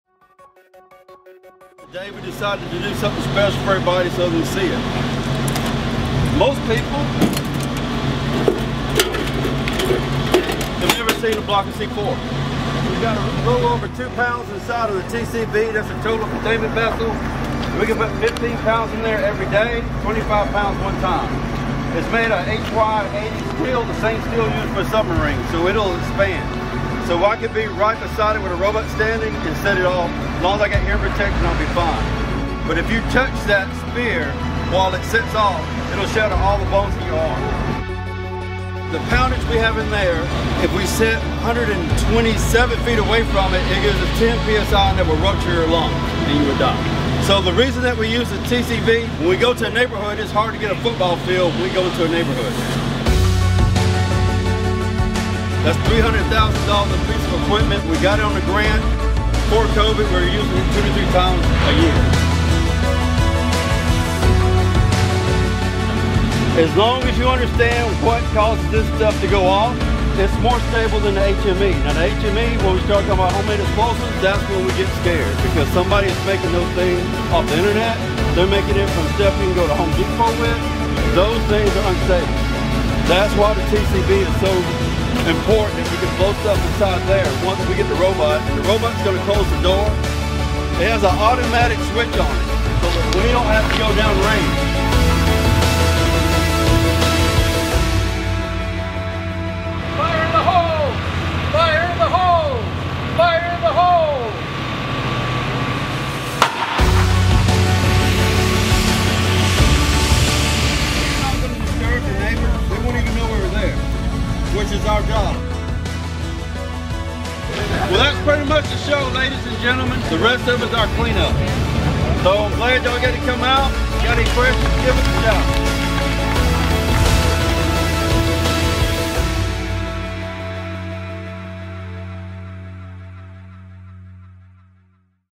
Blowing up C-4 in Charleston County Bomb Squad's TCV
The Charleston County Sheriff's Office Bomb Squad recently invited employees to witness a training exercise in which the team blew up more than 2 pounds of C-4 explosives inside the agency's Total Containment Vessel, or TCV. The tool, paid for with a grant, allows the squad to destroy explosives or potentially explosive devices at the scene of an incident without significant disruption to surrounding communities. Sgt. Carl Makins explained that to survive a blast the size of the one contained in the TCV for this exercise, a person without any protection would have to be about a football field away.